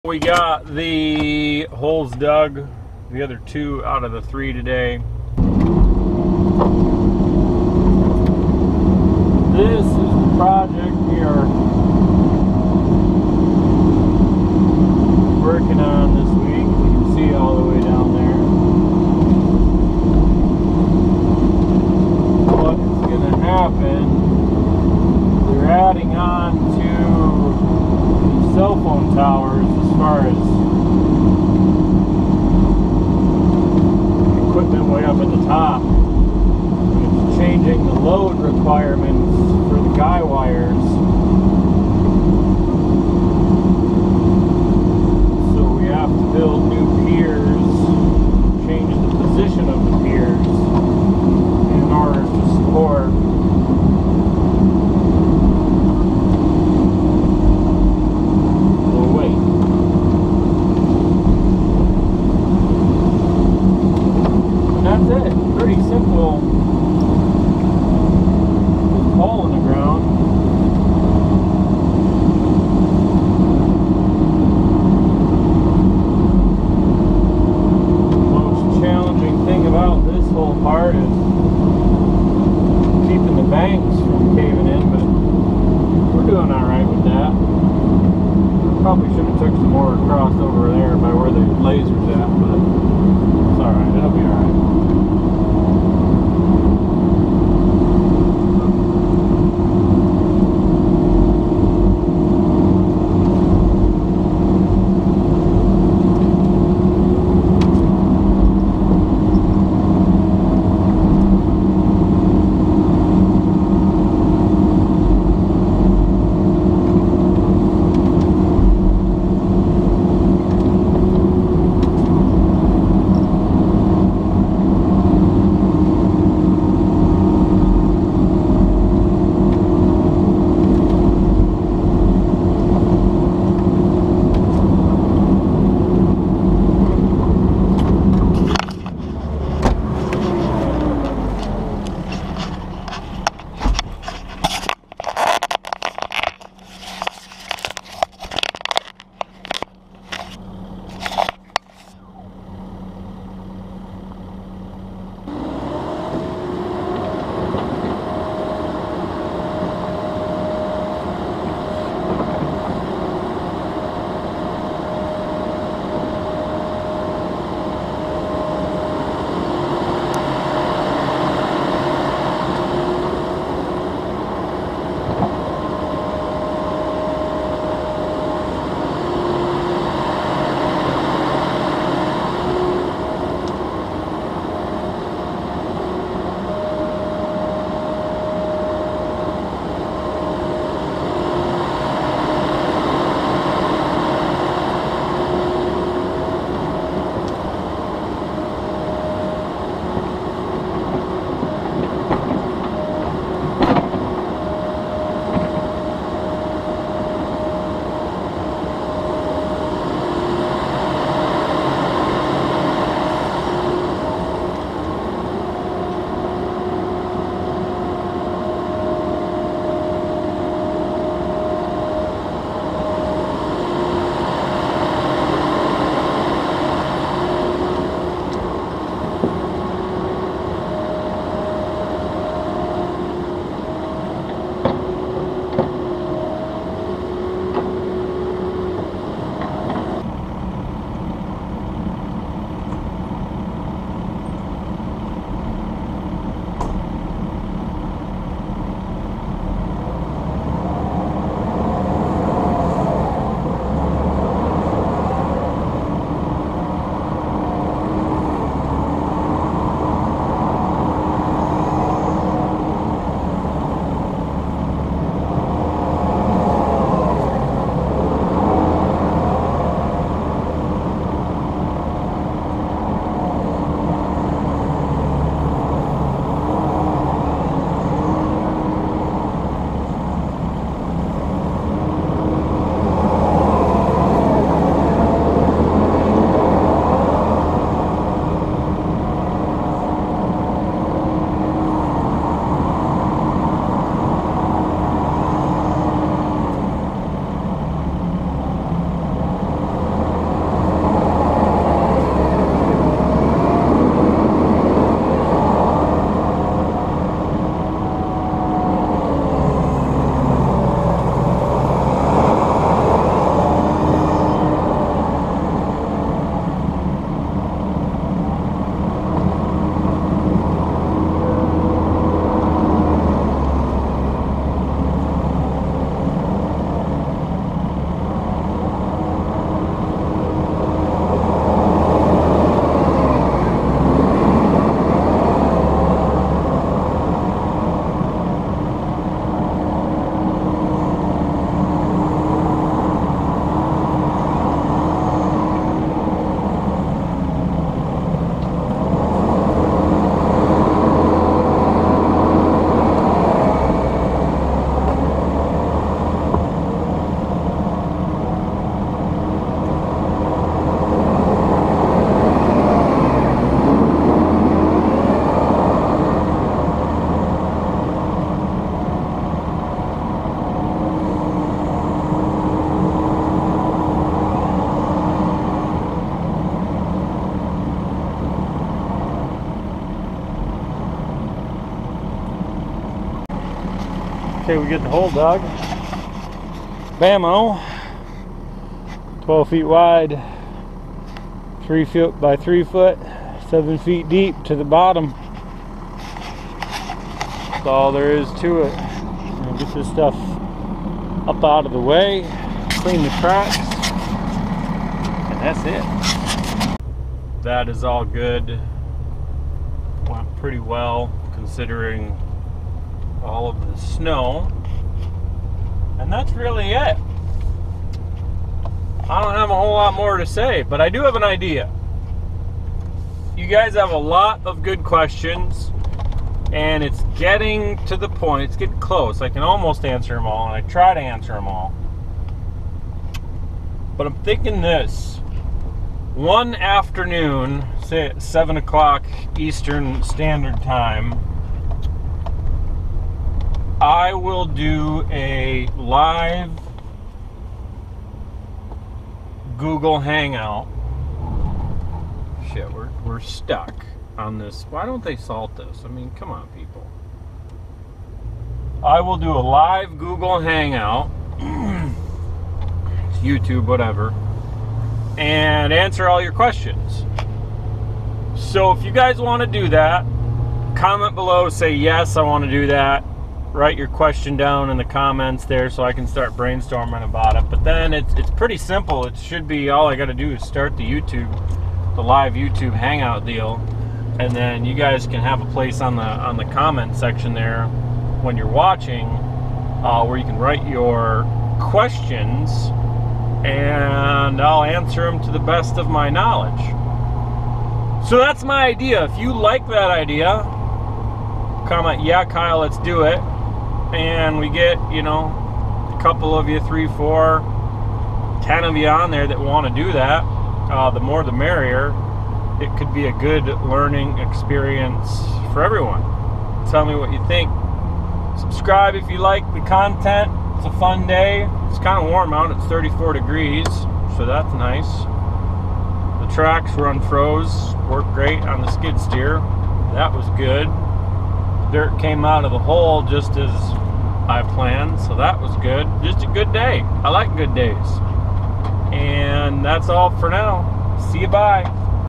Digging Holes for New Piers Do you Want a LIVE Q and A
Dug the other 2 holes today for the 3 new piers for the cell phone tower.

Do you want a live Q & A session where I can answer your questions?  Let me know if yes by commenting below. Thanks!